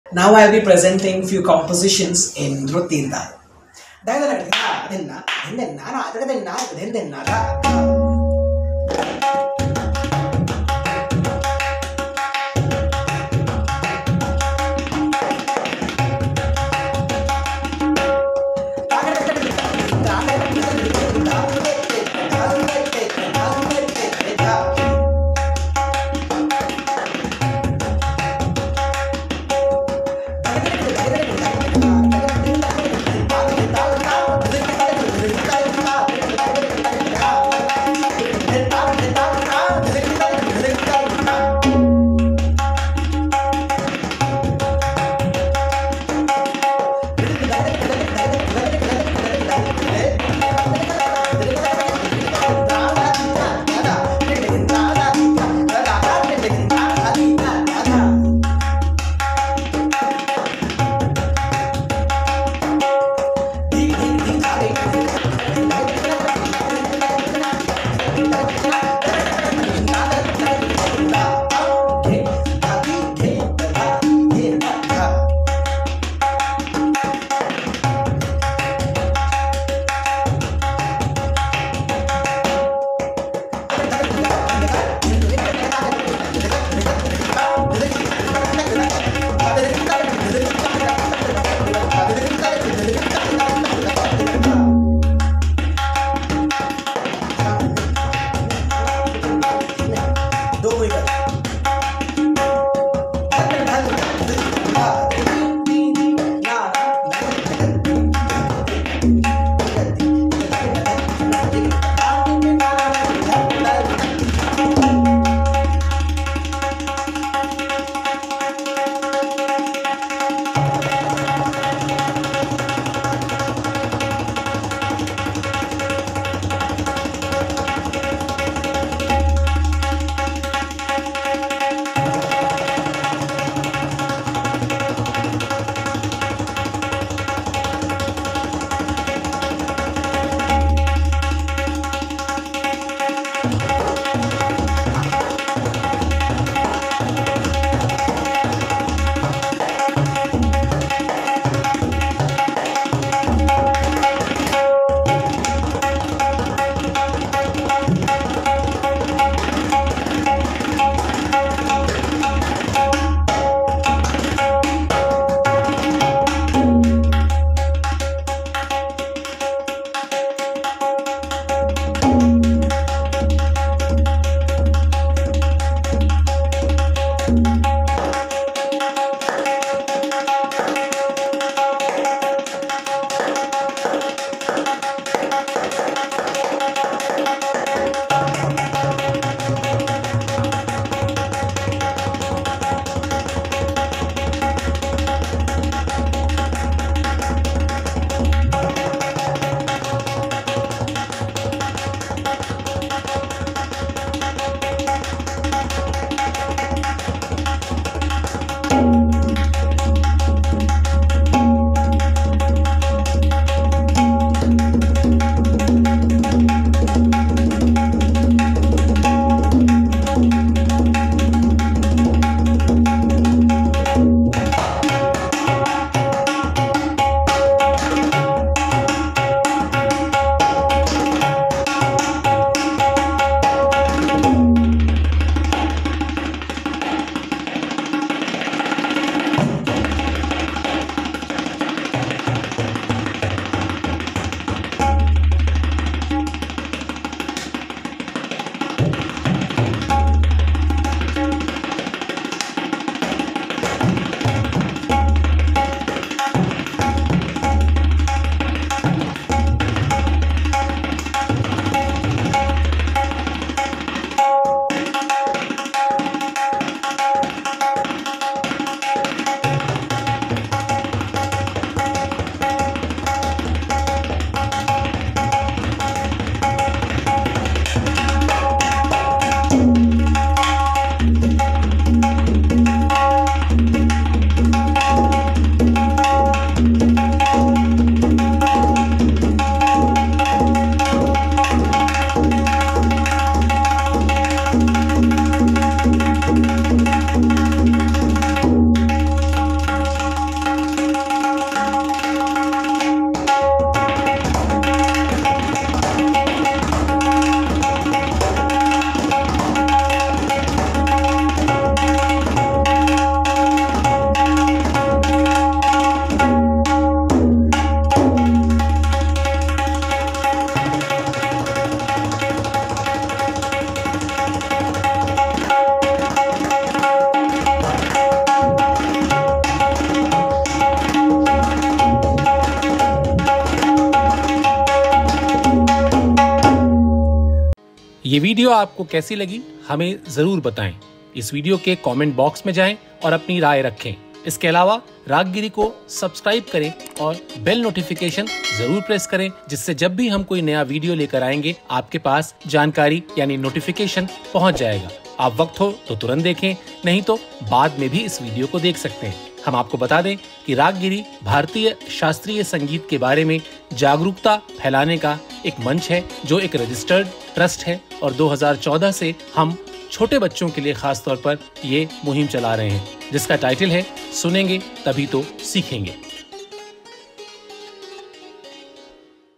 Chatur Lal's Spectacular Tabla Performance | A Rhythmic Masterpiece
Dive into the rhythmic brilliance of Chatur Lal, a maestro of the tabla. In this spectacular performance, Chatur Lal showcases his extraordinary talent and intricate tabla skills, captivating audiences with his dynamic rhythms and profound musicality. Experience the essence of Indian classical percussion through this unforgettable tabla performance.